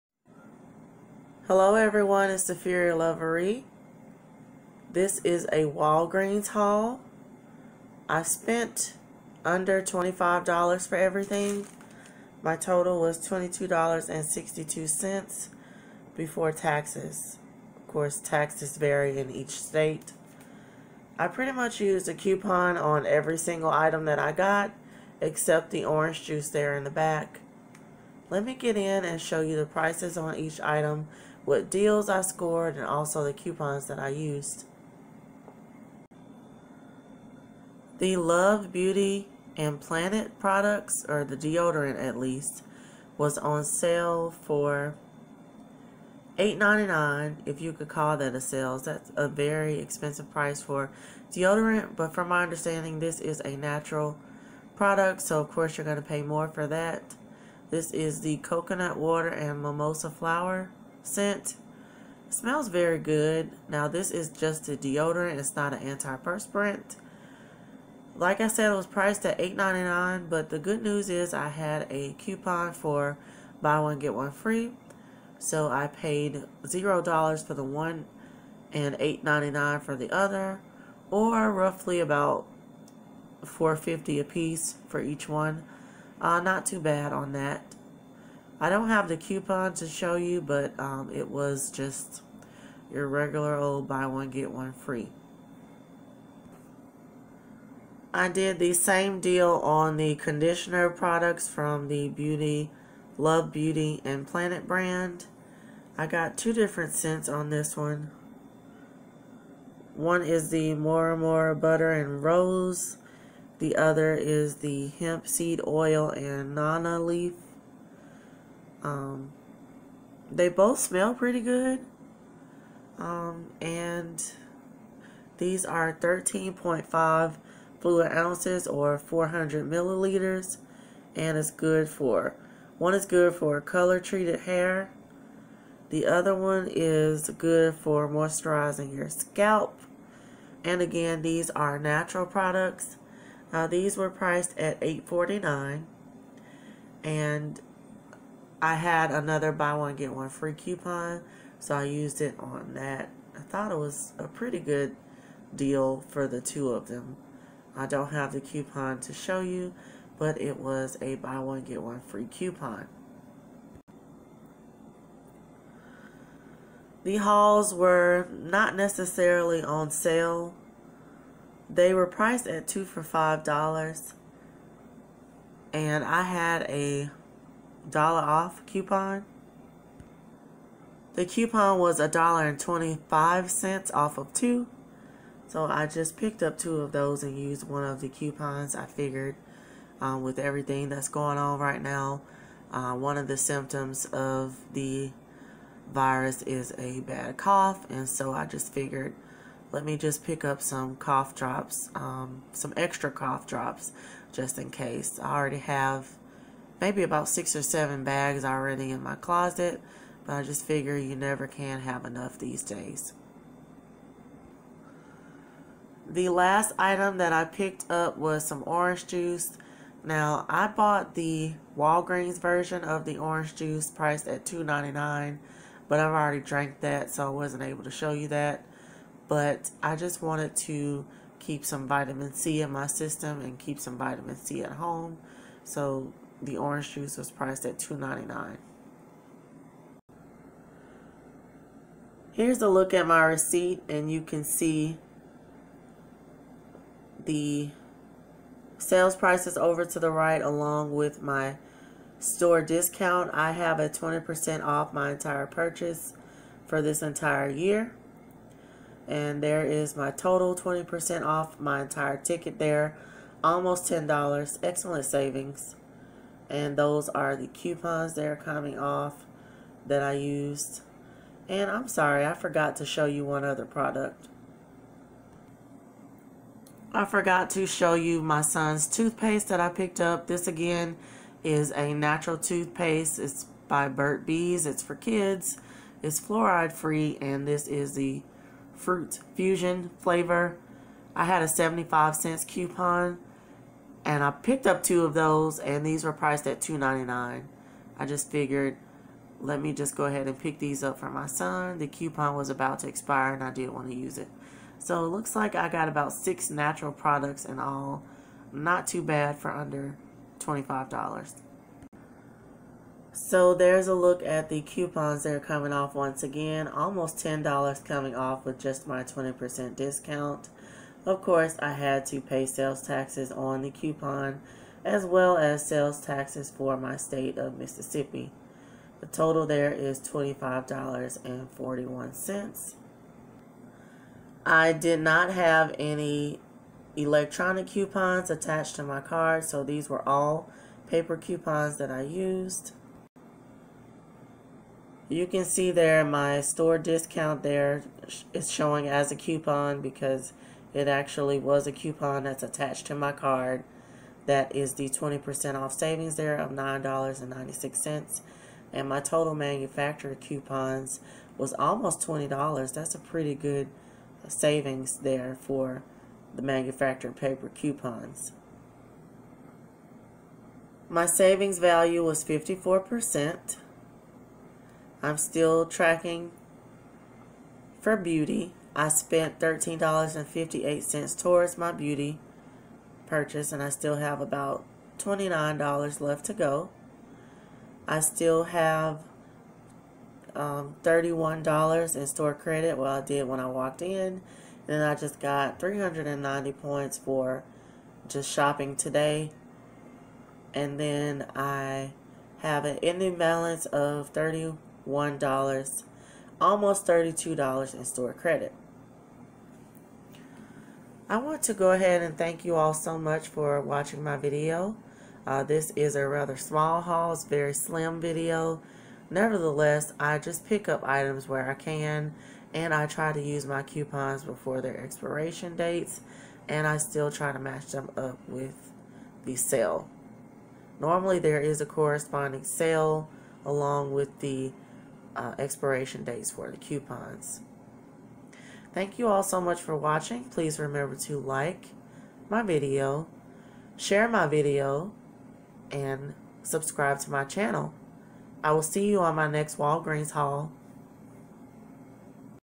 Walgreens Haul Under $25.00. Free Natural Skin & Hair Products. Great Deals.
Small but mighty haul.  Used $17.00 in coupons plus got $9.00 off my total purchase. Winning at the Walgreens!